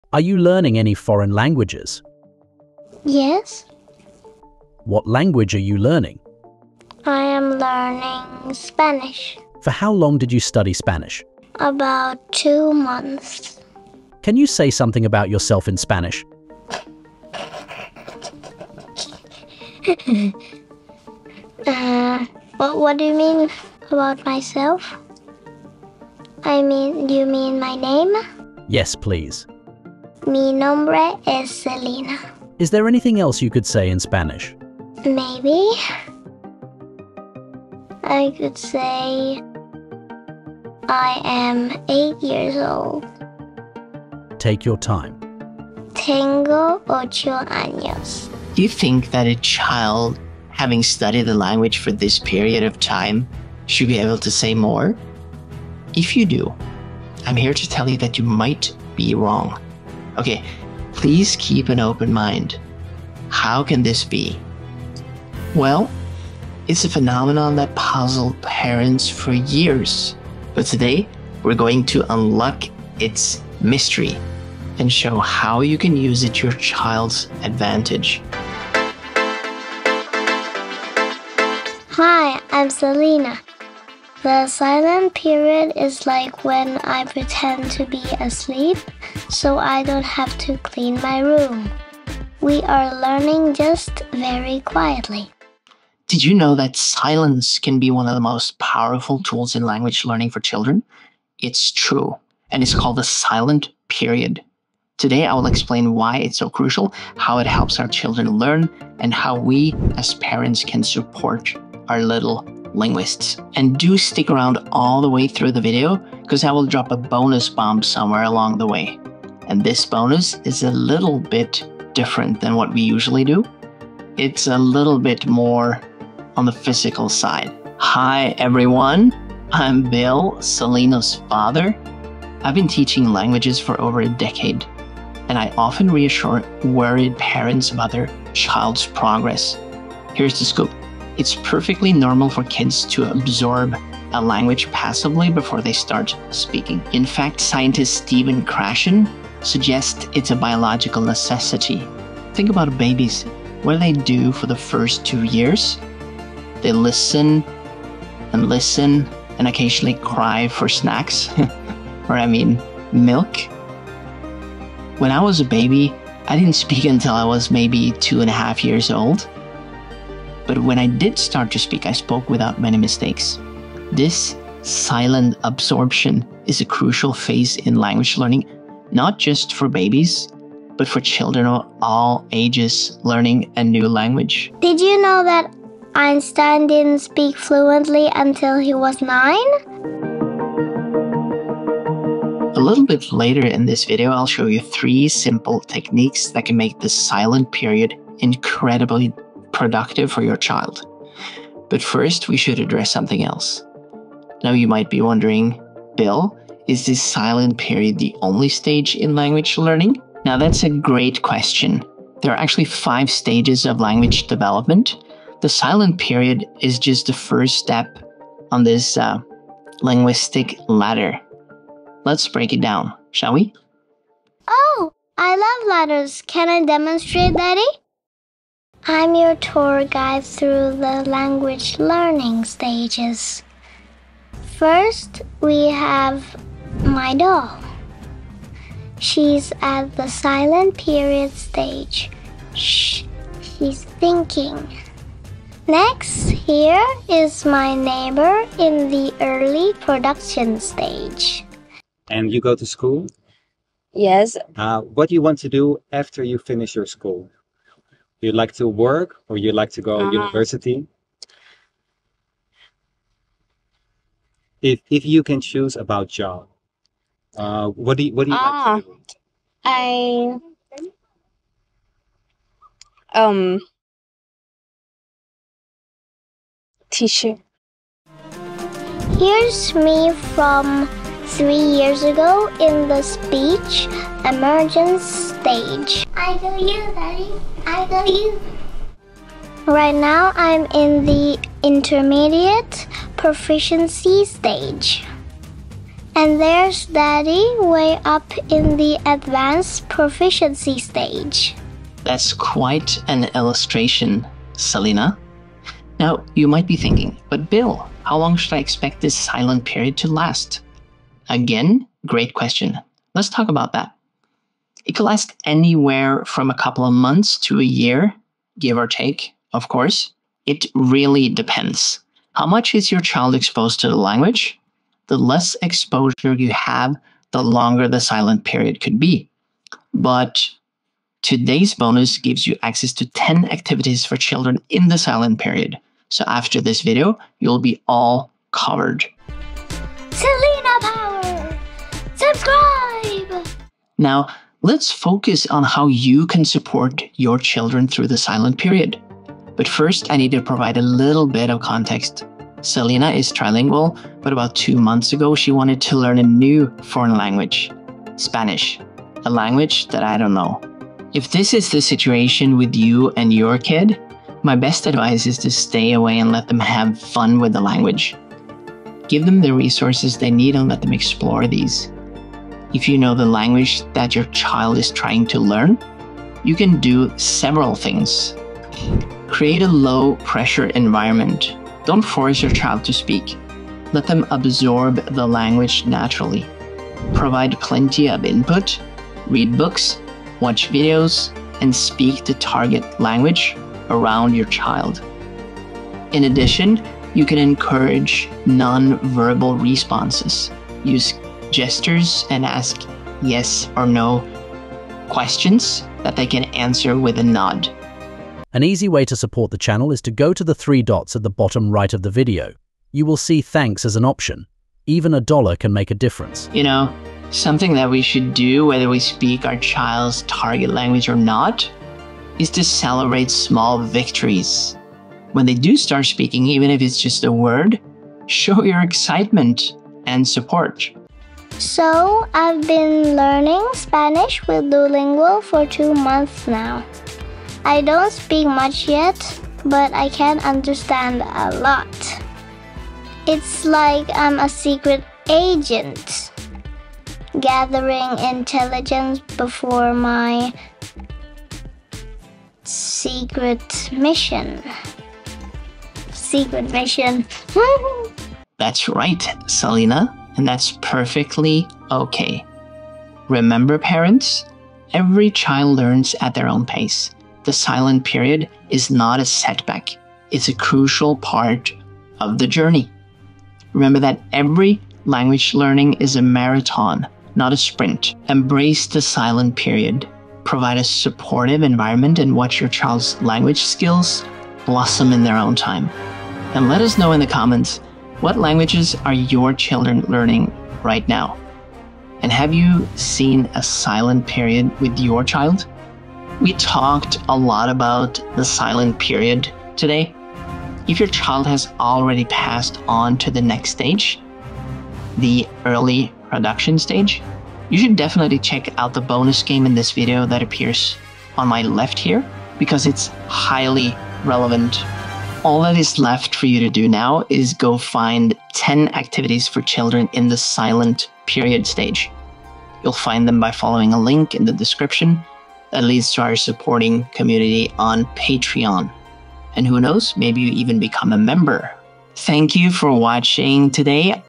A Surprising secret to Kids' foreign Language Success
Worried about your child's language progress? Discover why silence is golden in language learning! Uncover the power of the 'silent period' and learn how to support your child's linguistic journey. Gain expert insights, practical tips, and a special bonus to boost your child's language acquisition in this crucial stage of the learning.

10 ACTIVITIES FOR THE SILENT PERIOD (find the name of this video)

KIT FOR LANGUAGE LEARNING FOR YOUR KID

0:00 An Example of The Silent Period
0:48 Introduction to the topic
01:20 Selyna's perspective of the Silent Period
01:36 What you learn in this video
02:27 Background: "The Silent Period"
03:24 The five stages of language development
05:01 Details: The Silent Period
05:54 How to guide through The Silent Period
08:08 Final words

Are you concerned that your child isn't speaking enough in their new language? You're not alone. Many parents worry when their children seem to be making slow progress in language learning, especially during the crucial 'silent period.' In this comprehensive video, we delve deep into the fascinating world of language acquisition, focusing on the often misunderstood silent period and its vital role in your child's linguistic development.

Join Bill, an experienced language teacher and father, as he guides you through the intricacies of language learning stages. With over a decade of teaching experience with children, Bill shares valuable insights and practical advice for parents navigating their children's language learning journey. Discover why forcing your child to speak can actually hinder their progress and learn how to create a supportive environment that nurtures natural language acquisition.

Key topics covered in this video:

The Silent Period: Understand why this phase is a biological necessity in language learning.
Five Stages of Language Development: Get a clear overview of the linguistic ladder your child will climb.
Duration of the Silent Period: Learn how long to expect this phase to last and factors that influence its length.
Supporting Your Child: Discover three simple techniques to make the silent period incredibly productive.
Multilingual Development: Gain insights from Bill's experience raising his trilingual daughter, Selyna.
Common Concerns: Address worries about your child's language progress and learn why patience is key.
Expert Advice: Benefit from Bill's professional expertise in language teaching and personal experience as a parent.

Throughout the video, Bill's daughter Selyna adds a fun and relatable element, demonstrating language learning stages and sharing her own experiences as a young language learner. Her participation makes complex concepts accessible and entertaining for viewers of all ages.

This video is perfect for:

Parents of bilingual or multilingual children
Families considering language immersion programs
Educators working with young language learners
Anyone interested in the process of language acquisition

Don't miss out on the exclusive bonus offered in this video - a downloadable resource featuring 10 activities designed specifically for children in the silent period. These activities will help you engage your child in language learning without pressuring them to speak before they're ready.
By the end of this video, you'll have a new perspective on your child's language learning journey. You'll understand why silence doesn't mean lack of progress and how to effectively support your child through each stage of language development. Armed with this knowledge, you can create a positive, low-pressure environment that allows your child's language skills to flourish naturally.
Remember, language learning is a marathon, not a sprint. This video will equip you with the tools and understanding to support your child's linguistic growth, celebrate their progress (no matter how small), and embrace each stage of their journey to multilingualism.
Share your experiences and questions in the comments - we'd love to hear about your family's language learning adventures!